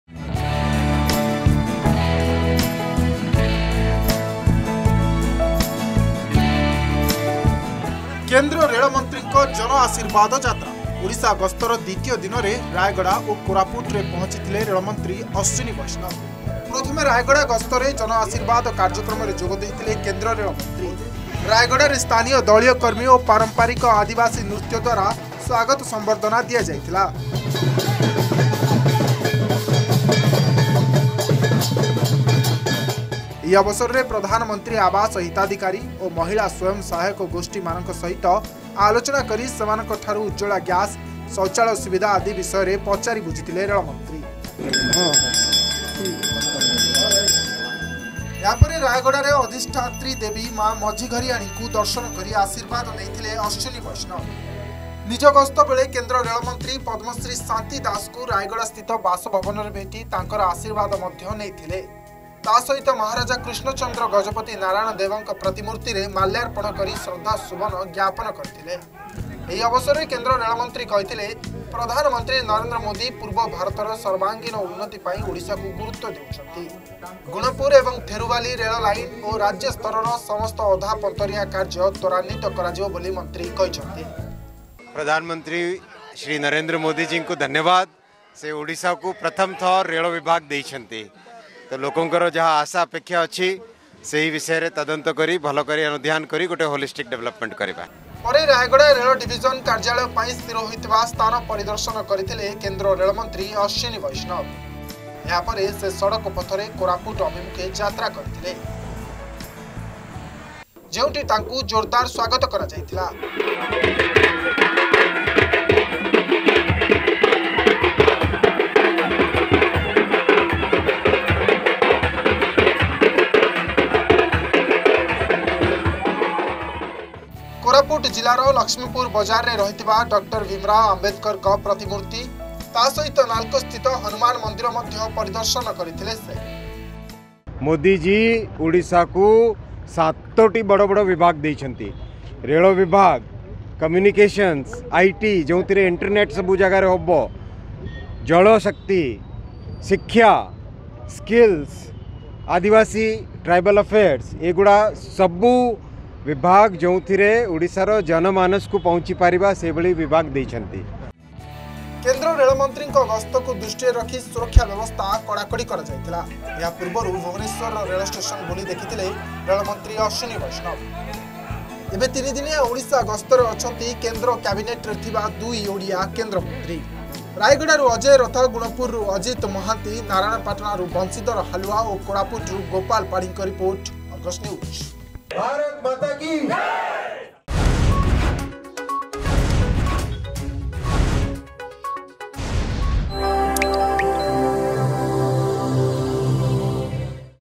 ରାୟଗଡା ପରେ କୋରାପୁଟ ଗସ୍ତ କରିଛନ୍ତି କେନ୍ଦ୍ର ରେଳ ମନ୍ତ୍ରୀ ଅଶ୍ବିନୀ ବୈଷ୍ଣବ।
କେନ୍ଦ୍ର ରେଳମନ୍ତ୍ରୀଙ୍କ ଜନ ଆଶୀର୍ବାଦ ଯାତ୍ରା । ଓଡ଼ିଶା ଗସ୍ତର ଦ୍ବିତୀୟ ଦିନରେ ରାୟଗଡା ଓ କୋରାପୁଟରେ ପହଂଚିଥିଲେ ରେଳମନ୍ତ୍ରୀ ଅଶ୍ୱିନୀ ବୈଷ୍ଣବ ।  ପ୍ରଥମେ ରାୟଗଡା ଗସ୍ତରେ ଜନ ଆଶୀର୍ବାଦ କାର୍ଯ୍ୟକ୍ରମରେ ଯୋଗଦେଇଥିଲେ କେନ୍ଦ୍ର ରେଳମନ୍ତ୍ରୀ । ରାୟଗଡରେ ସ୍ଥାନୀୟ ଦଳୀୟ କର୍ମୀ ଓ ପାରମ୍ପରିକ ଆଦିବାସୀ ନୃତ୍ୟ ଦ୍ୱାରା ସ୍ୱାଗତ ସମ୍ବର୍ଧନା ଦିଆଯାଇଥିଲା ।